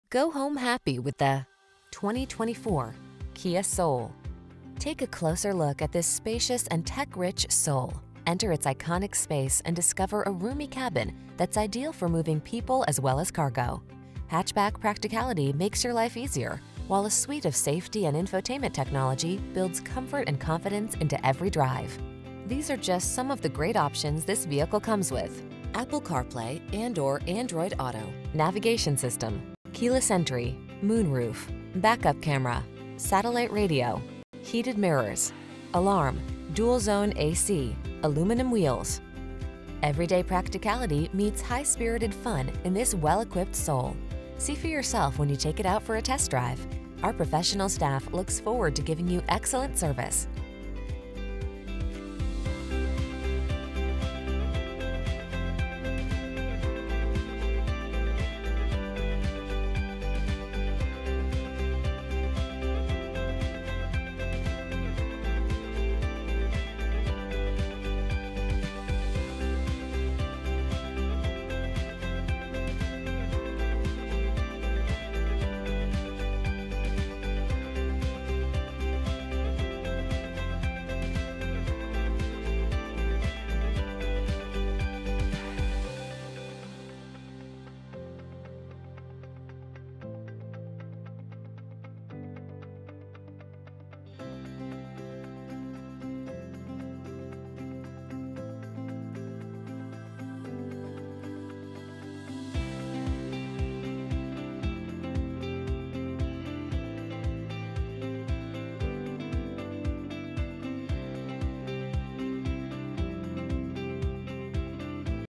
2024 Kia Soul Highland Park, Northbrook, Skokie, Evanston, North Chicago K50927
Snow White Pearl New 2024 Kia Soul GT-Line available in 250 Skokie Valley Road, Highland Park, IL at McGrath Kia of Highland Park.  Servicing the Highland Park, North Brooke, Skokie, Evanston, Chicago, IL area.